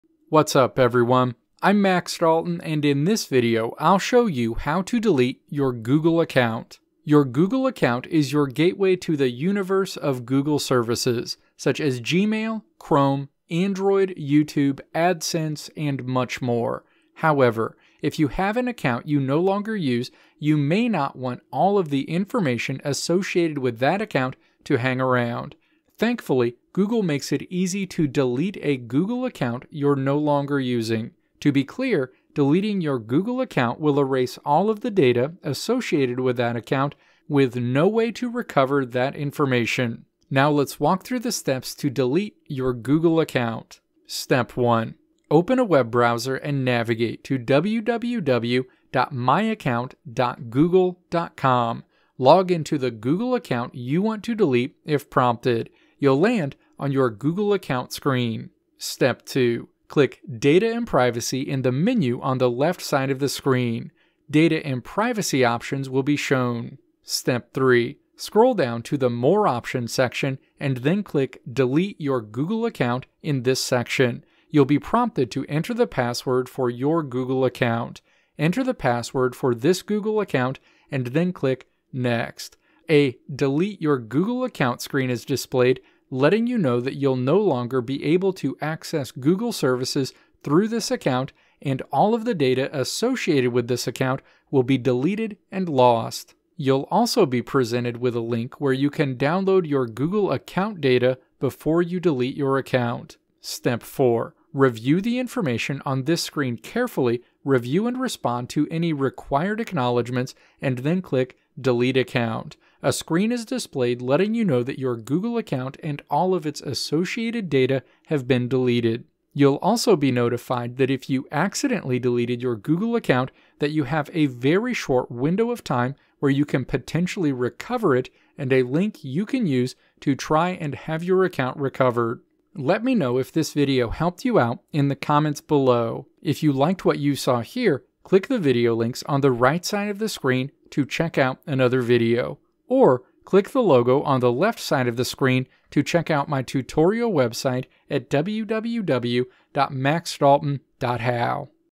How to Delete Google Account
In this video I'll show you how to delete your Google account.

Timestamps:
Introduction: 0:00
Steps to Delete Google Account: 0:40
Conclusion: 2:12

Video Transcript:
Your Google account is your gateway to the universe of Google services, such as Gmail, Chrome, Android, YouTube, AdSense, and much more. However, if you have an account you no longer use, you may not want all of the information associated with that account to hang around. Thankfully, Google makes it easy to delete a Google account you're no longer using. To be clear, deleting your Google account will erase all of the data associated with that account, with no way to recover that information.

Now let's walk through the steps to delete your Google account.

Step 1. Log into the Google account you want to delete if prompted. You'll land on your Google Account screen.

Step 2. Click "Data and Privacy" in the menu on the left side of the screen. Data and Privacy options will be shown.

Step 3. Scroll down to the More Options section, and then click "Delete Your Google Account" in this section. You'll be prompted to enter the password for your Google account. Enter the password for this Google account, and then click "Next." A Delete Your Google Account screen is displayed, letting you know that you'll no longer to be able to access Google services through this account, and all of the data associated with this account will be deleted and lost. You'll also be presented with a link where you can download your Google account data before you delete your account.

Step 4. Review the information on this screen carefully, review and respond to any required acknowledgements, and then click "Delete Account." A screen is displayed letting you know that your Google account and all of its associated data have been deleted. You'll also be notified that if you accidentally deleted your Google account that you have a very short window of time where you can potentially recover it, and a link you can use to try and have your account recovered.